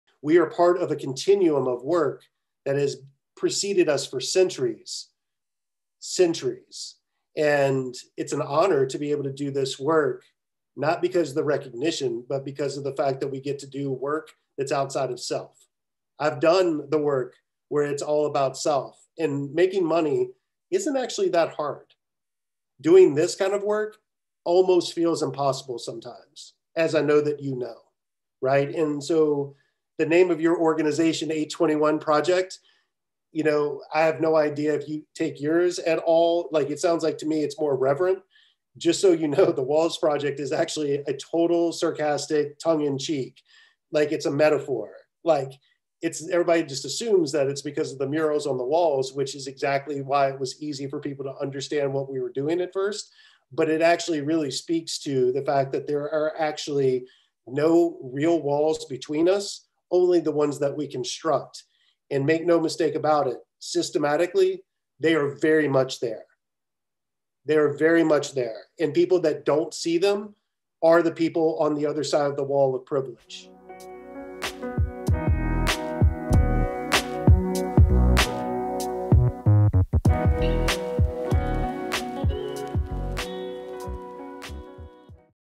WATCH THURSDAY (3/25/2021): Expanding Our Roots With Casey Phillips
Our next "Expanding Our Roots" interview with Casey Phillips from The Walls Project and Futures Fund will be posted this Thursday!

Casey Phillips describes himself as a creative futurist with 20+ years of experience forecasting and launching entertainment, technology, and art brands. He is a prominent brand strategist specializing in the cultural economy and nonprofit sector by implementing innovative strategies based on global best practices, data analysis, and future market trends. He is also a proponent of creative placemaking and smart growth principles to reactivate communities through collaborative community development. Casey is the Founder of The Force Agency in Los Angeles, California, and Co-Founder/Director of The Walls Project 501c3 in Baton Rouge, Louisiana.

To learn more about The Walls Project and The Futures Fund, go